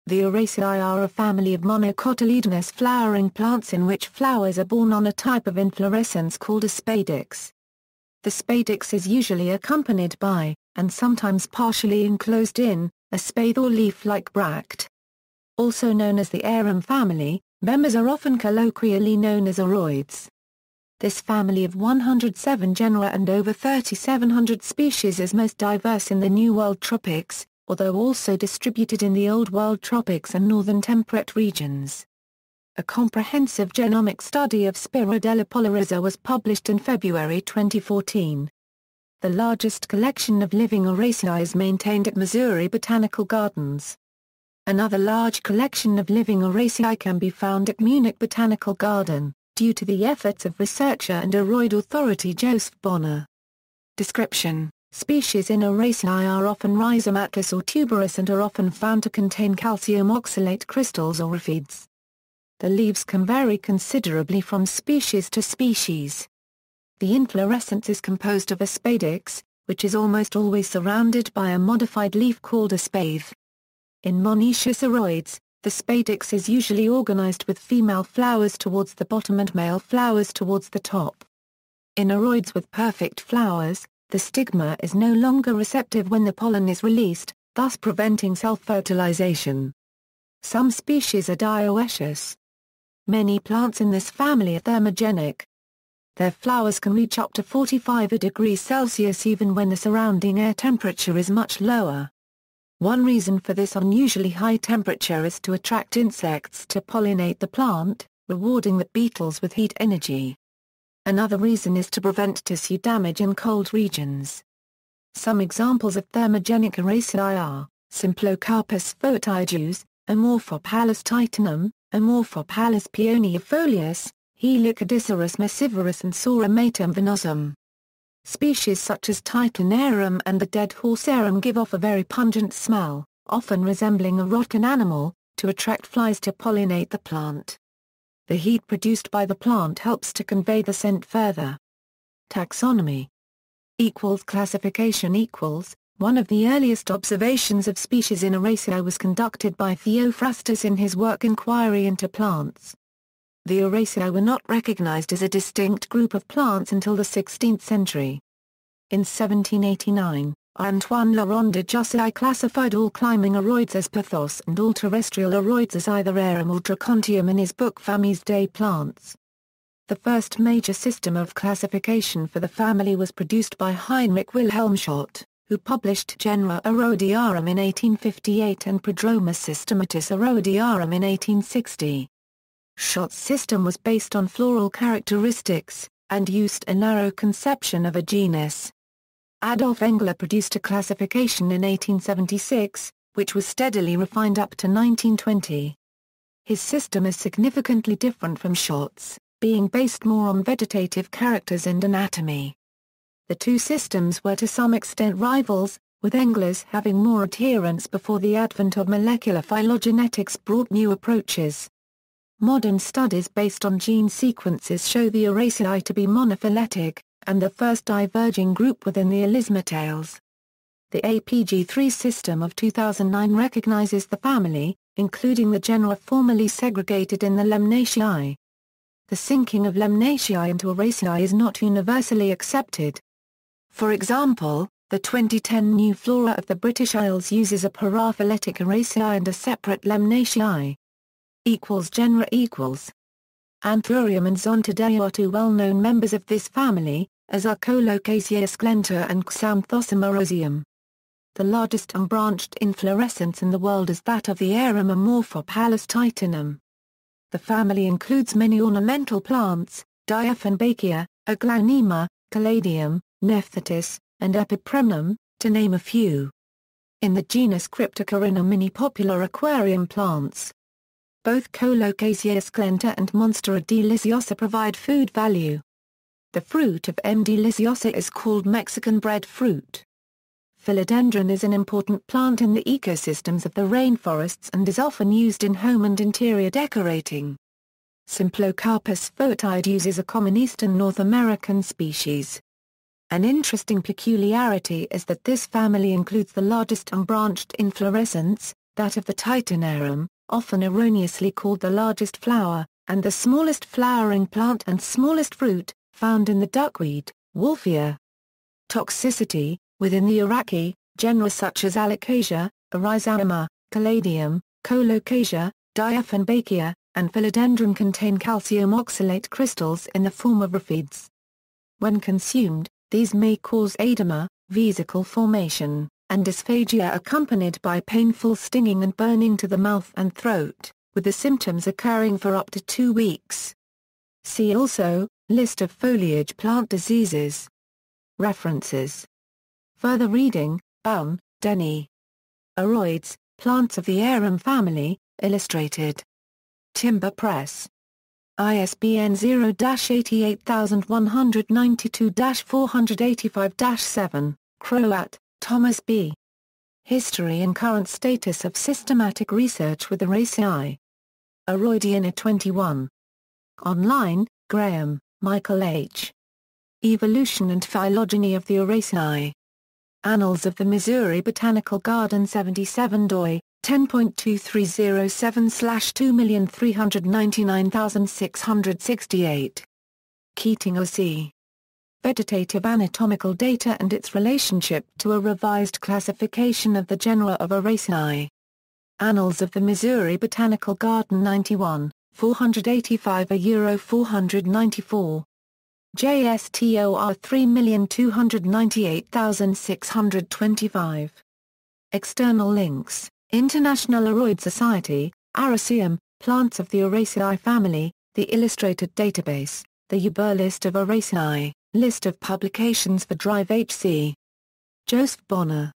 Araceae
The Araceae are a family of monocotyledonous flowering plants in which flowers are borne on a type of inflorescence called a spadix. The spadix is usually accompanied by, and sometimes partially enclosed in, a spathe or leaf-like bract. Also known as the arum family, members are often colloquially known as aroids. This family of 107 genera and over 3700 species is most diverse in the New World tropics, although also distributed in the Old World tropics and northern temperate regions. A comprehensive genomic study of Spirodela polyrhiza was published in February 2014.
The largest collection of living Araceae is maintained at Missouri Botanical Gardens. Another large collection of living Araceae can be found at Munich Botanical Garden, due to the efforts of researcher and aroid authority Josef Bogner.